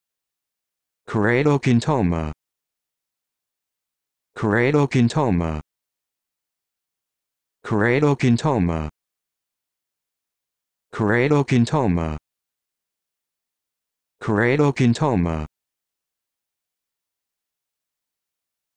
How to Pronounce Keratoacanthoma
This video shows you how to pronounce Keratoacanthoma